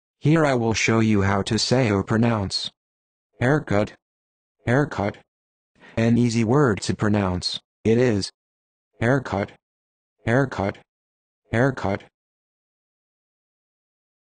How to Say or Pronounce Haircut
This video shows you how to say or pronounce Haircut.

How accurate does it say Haircut? How would you say Haircut?